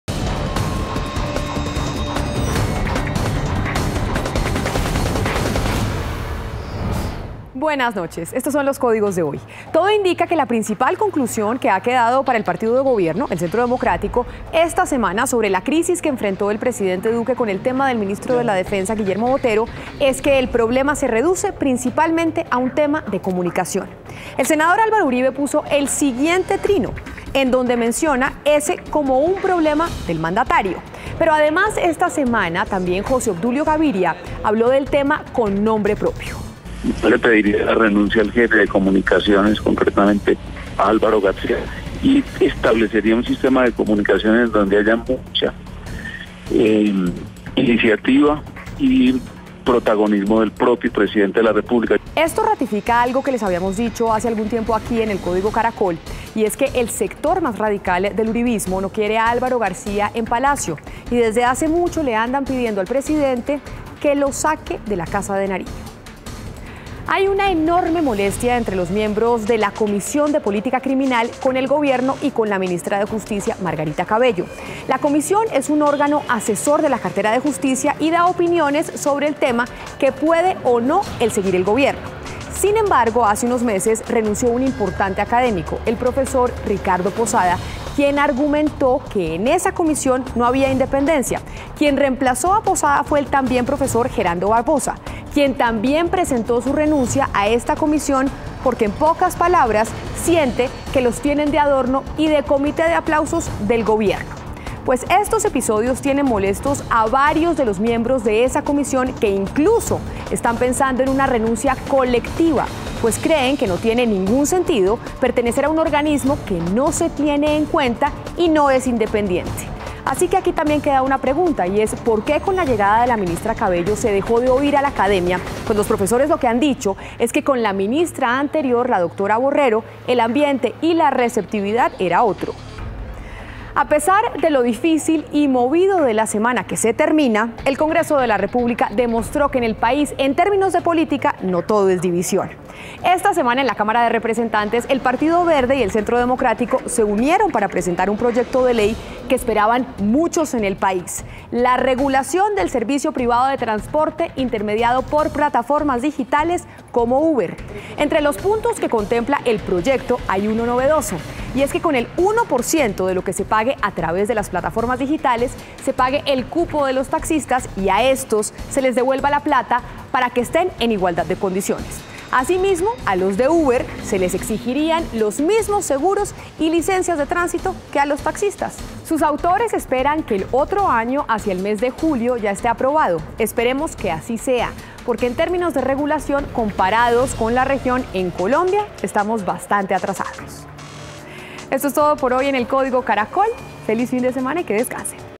Centro Democrático y Partido Verde presentan proyecto para regular Uber, Beat y otras aplicaciones
Ambos partidos se aliaron en la Cámara de Representantes para proponer la regulación del servicio privado de transporte a través de plataformas digitales.